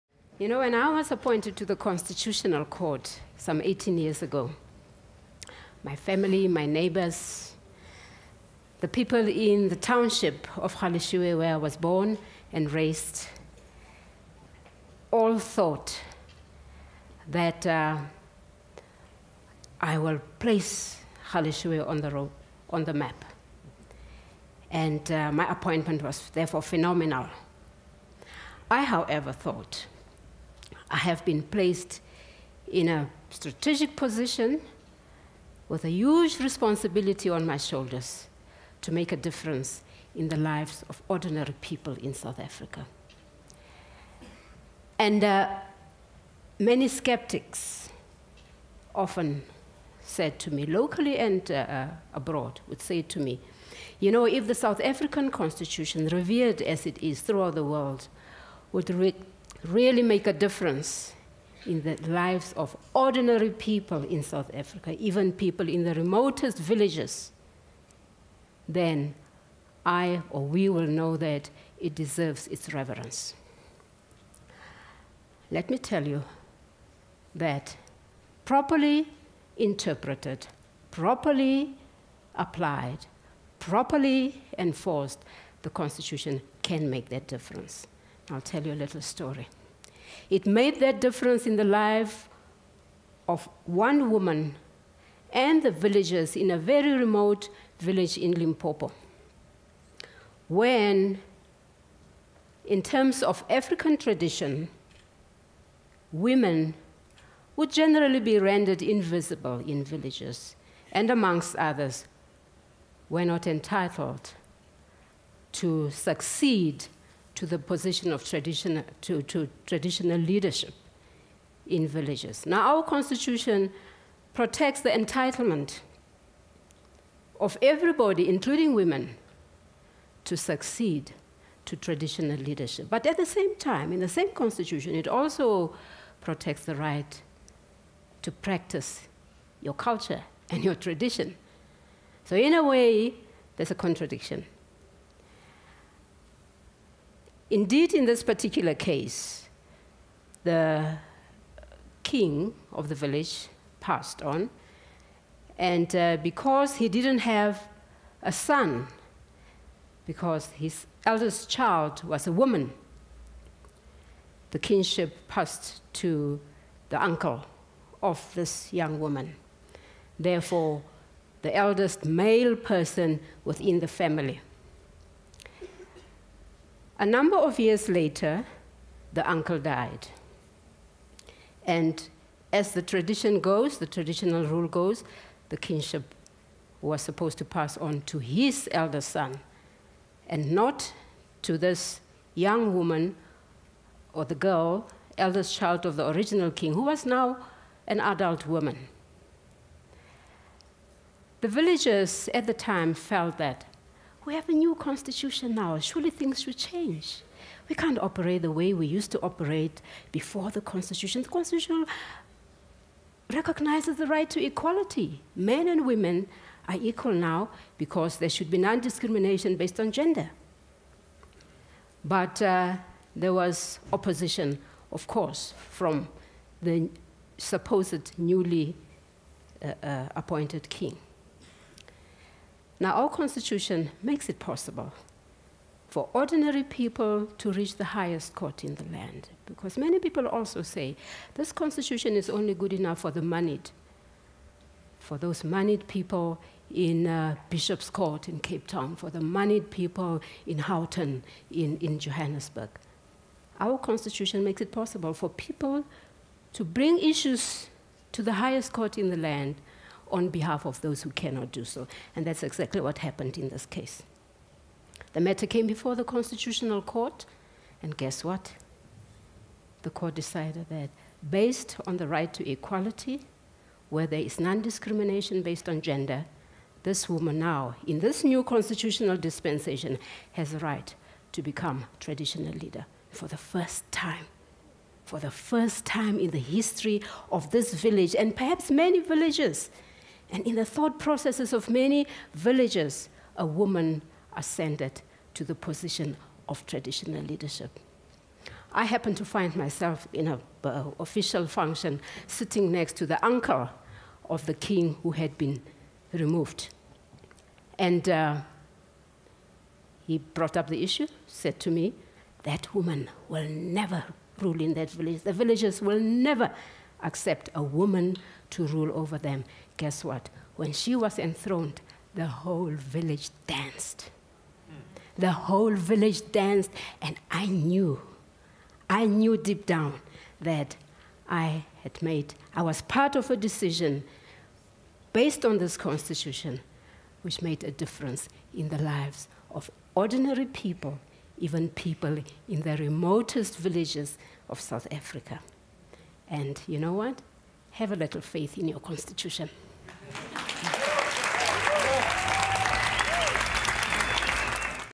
Yvonne Mokgoro: One woman's constitutional right to rule
Since Yvonne Mokgoro's 1994 appointment to the Constitutional Court of South Africa by Nelson Mandela, South Africa's first black female judge has devoted her life to restoring justice to ordinary people.